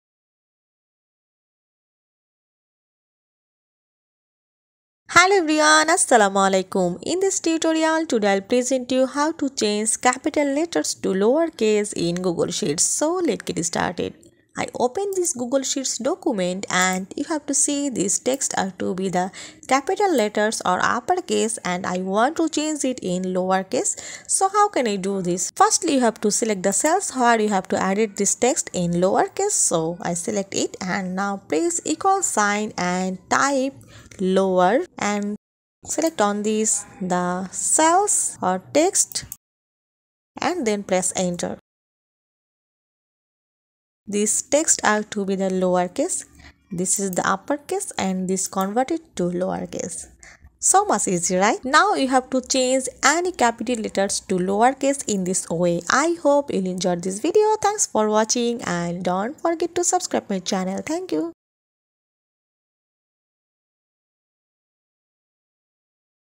How to change capital letters to lowercase in Google Sheets
Hello everyone, Today I will present you, How to change capital letters to lowercase in Google Sheets.